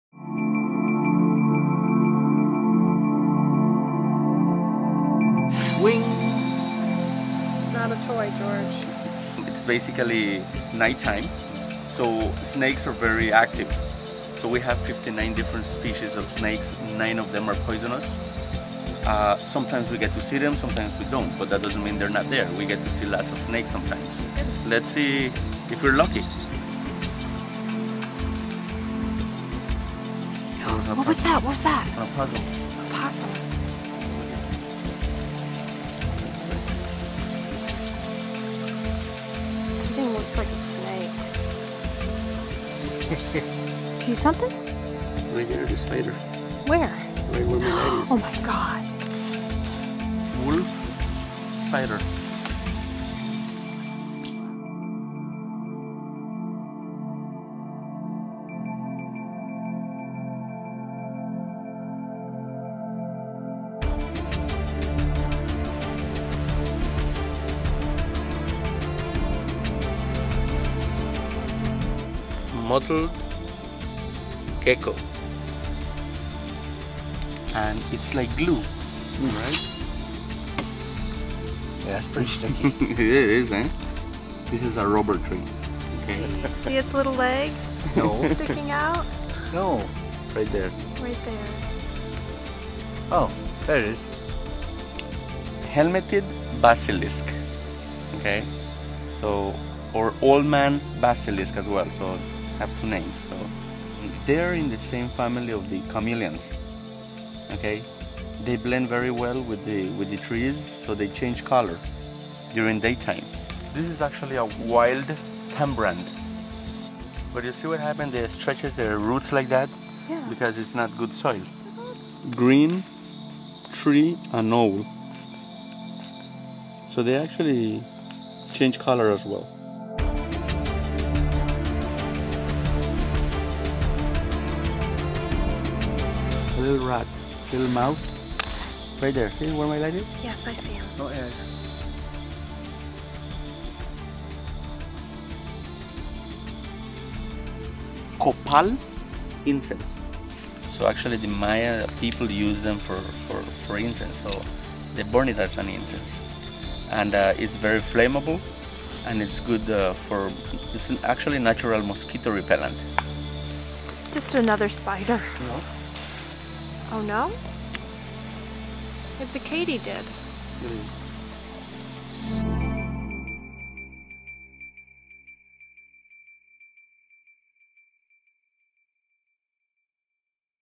Night Hike at Sleeping Giant Resort
Sleeping Giant Resort in the Mayan Mountains of Belmopan, Belize
TRT:  3:00